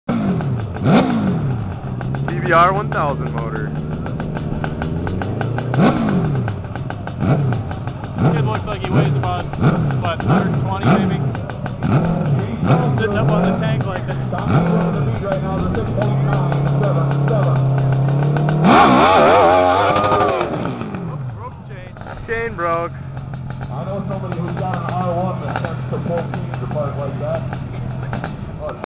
red wing hill climb 3 May 09
cbr 1000 powered hill climber! dude said he broke a chain in practice the day before and sure enough the chain lost round two to the cbr motor.